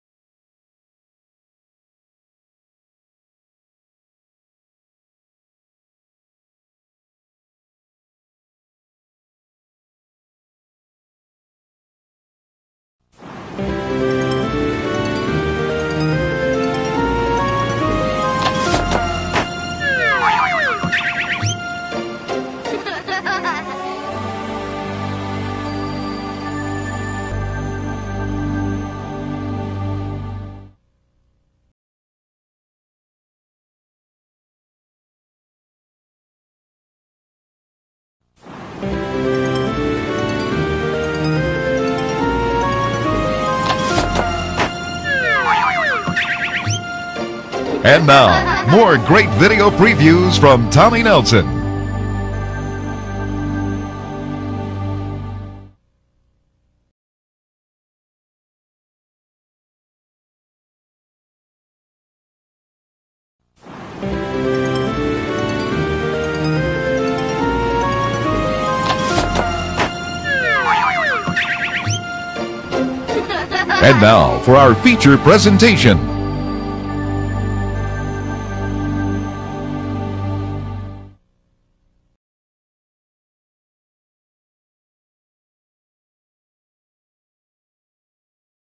All 3 Tommy Nelson logo variants (1997-2005) RARE! (First Video of 2023!)
These are all the Tommy Nelson VHS logos from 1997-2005 that I made by myself with the logos fully played.

0:00 Intro
0:12 Original Version
0:38 More Great Video Previews
1:03 Feature Presentation